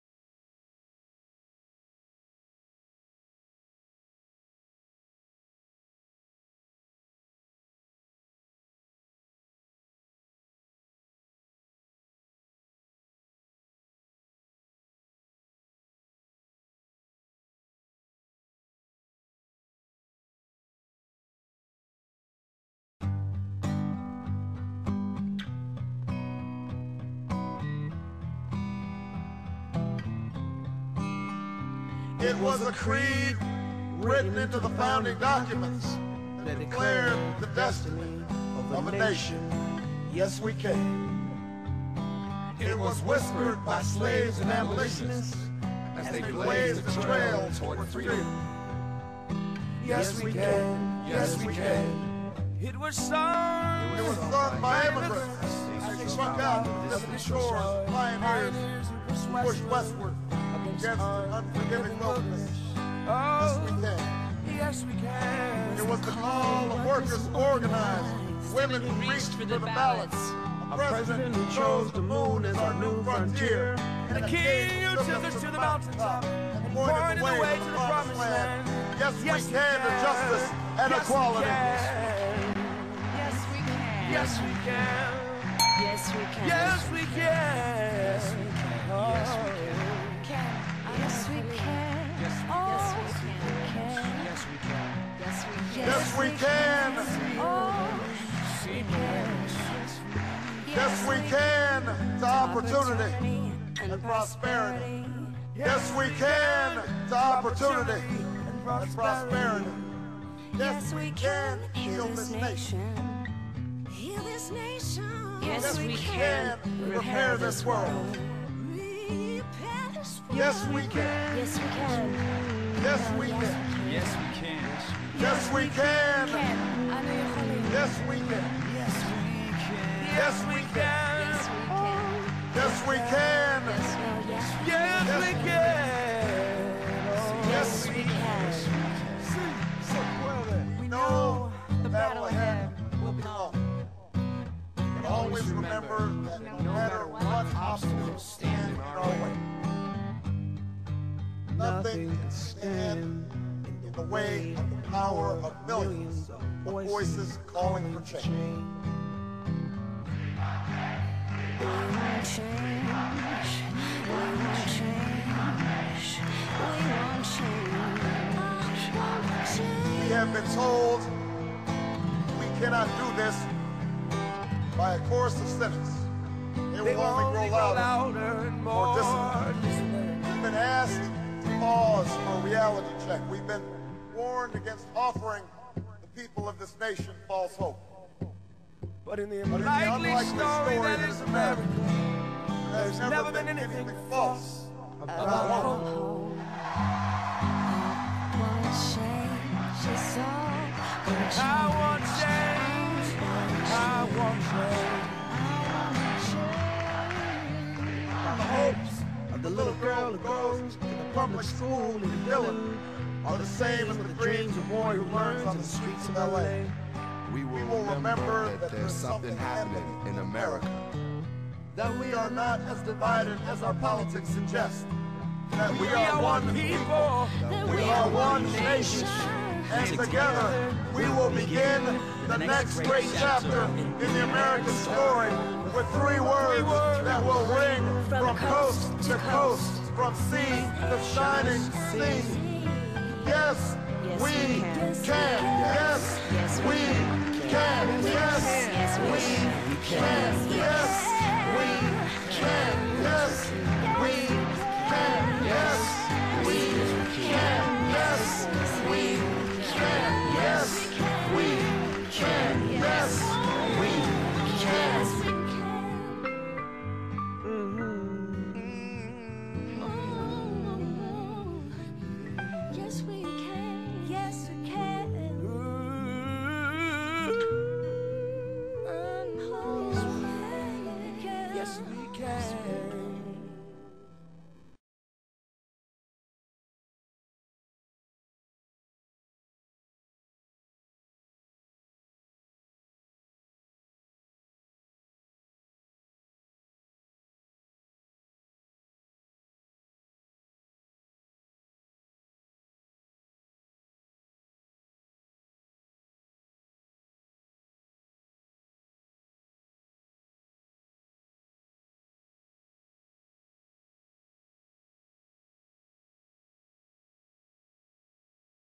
Animated Electoral Map
This is an animation of the 2008 presidential election electoral map taken from poll data from June-October.